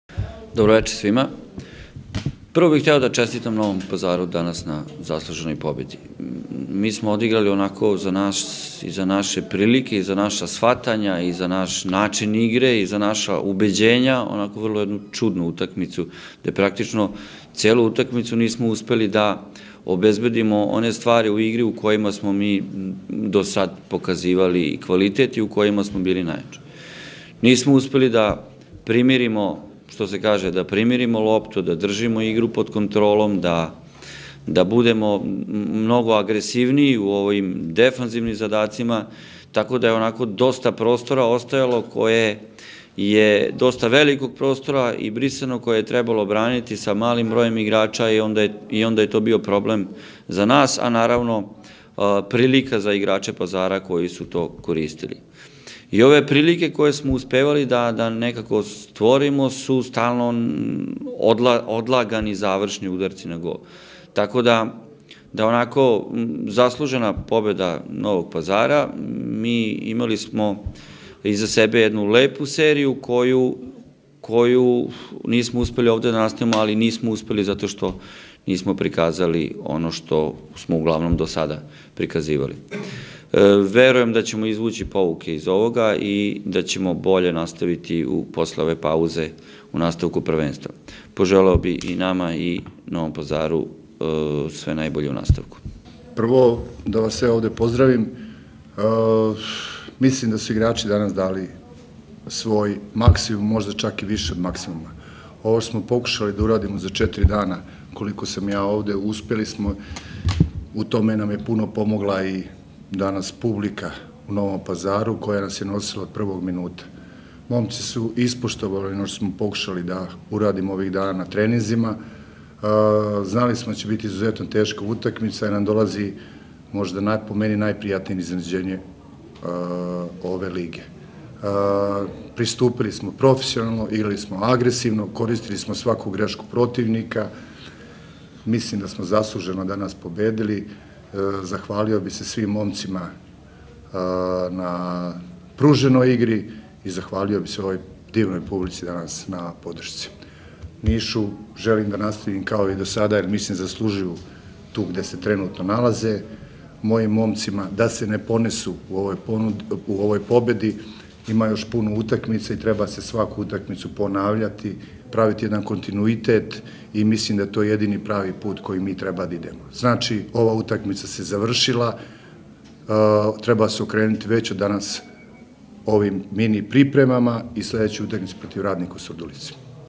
Govedarica i Rastavac posle utakmice u Novom Pazaru
Posle poraza u Novom Pazaru trener niškog Radničkog Milan Rastavac pomalo razočarano konstatovao je da njegov tim nije prikazao ništa od onoga što ga je krasilo tokom dosadašnjeg toka šampionata. Sa druge strane, Zoran Govedarica debitovao je na klupi Novog Pazara pobedom i zbog toga nije krio zadovoljstvo